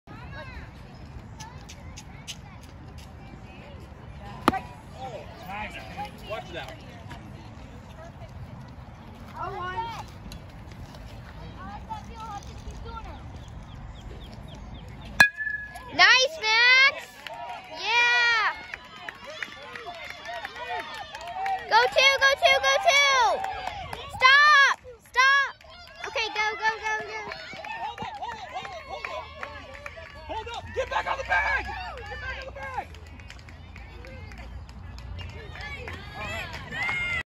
Max’s triple.
Baseball game. 5/15/22. Little buddy had two triples.